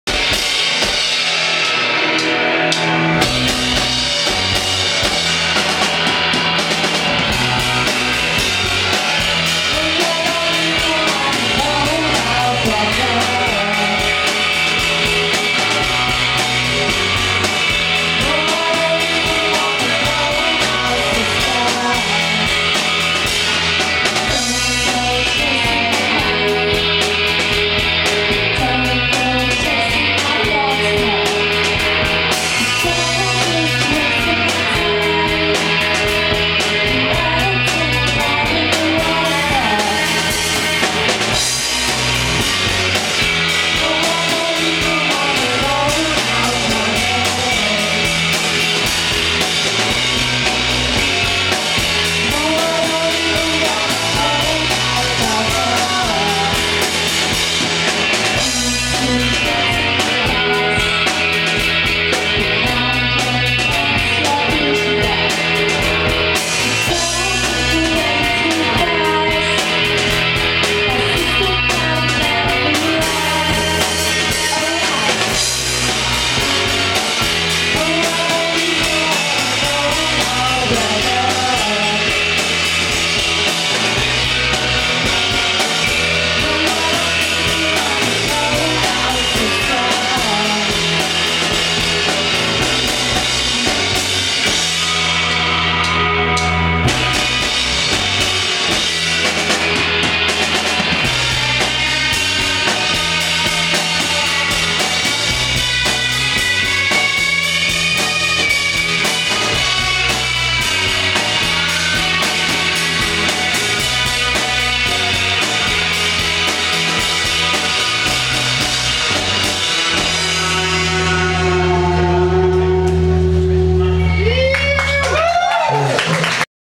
The Orielles live @ The Victoria, Dalston, London, 26/09/15 (Part 2)
The Orielles performing live for Idaho #3 at The Victoria, Dalston, London on September 26th, 2015.